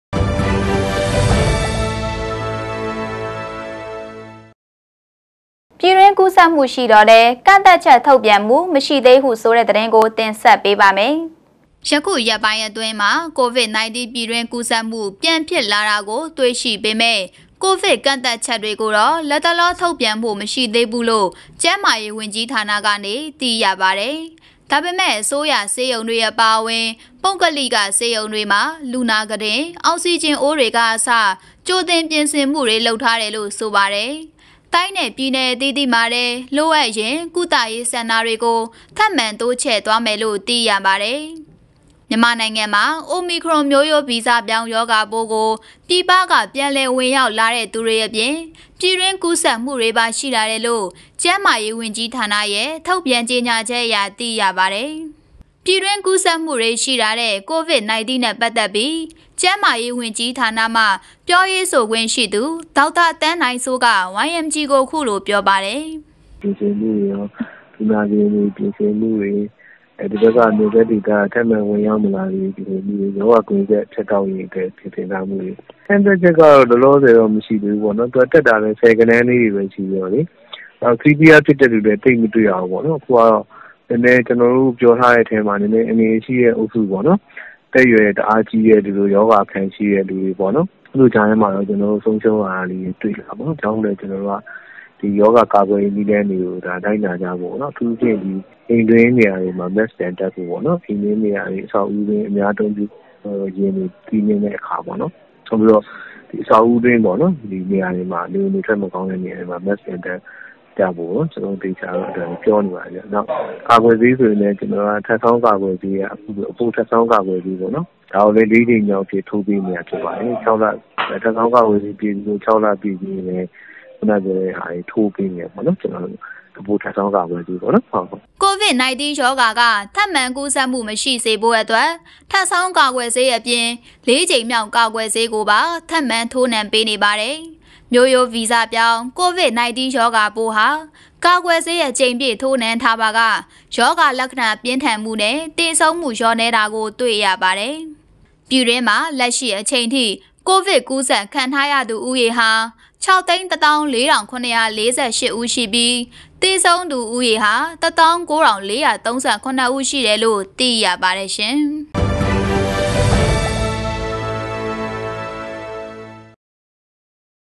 ပြည်တွင်းကူးစက်မှုရှိသော်လည်း ကန့်သတ်ချက်ထုတ်ပြန်ဖို့ မရှိသေးဟုဆို
ယခုရက်ပိုင်းအတွင်းမှာ ကိုဗစ်- ၁၉ ပြည်တွင်းကူးစက်မှု ပြန်ဖြစ်လာတာကိုတွေ့ရှိပေမဲ့ ကိုဗစ်ကန့်သတ်ချက်တွေကိုတော့ လတ်တလော ထုတ်ပြန်ဖို့မရှိသေးဘူးလို့ ကျန်းမာရေးဝန်ကြီးဌာနကနေ သိရပါတယ်။